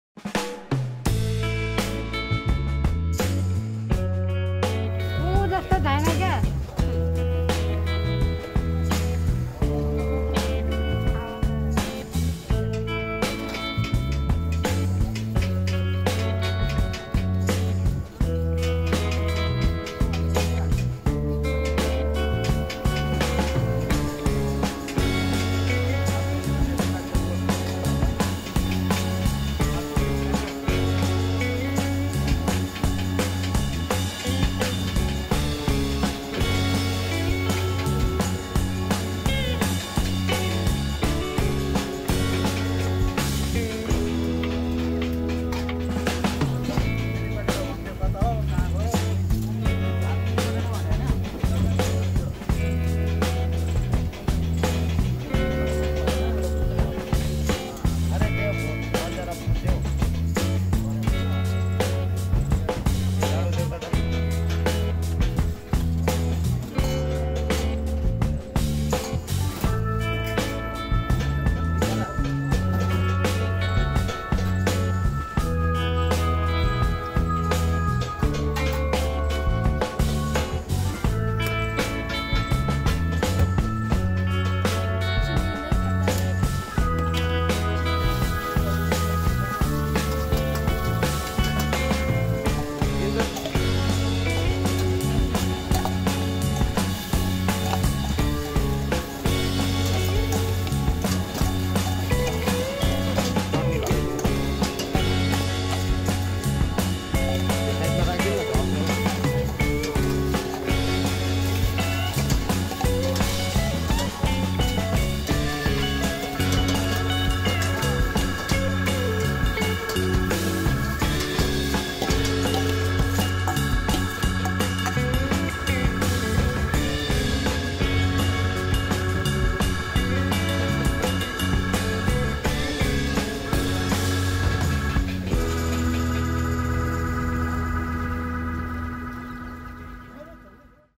Literally the best momos in Gurgaon
I will never forget Huda Street Food Market in Gurugram.
These tandoori momos (and the kurkure momos from another video) were the best momos I ever ate. Take a look!

Location:
C4G2+M22, Block B, Sector 56, Gurugram, Haryana 122011

This is an interesting place, with street food vendors only. Just a few other vendors with vegetables and fruits - but still food. The opening time is after 4:00 pm. The location is not close to anything in walking distance. People come here particularly to eat. Some of them are ordering and eating in the car, like in a 'drive-in'. They park near the vendors and they get served without getting out of the car.

After eating here some momos, I perfectly understand why so many people come here to eat. The food is awesome. If you are in Gurgaon, do not miss this place.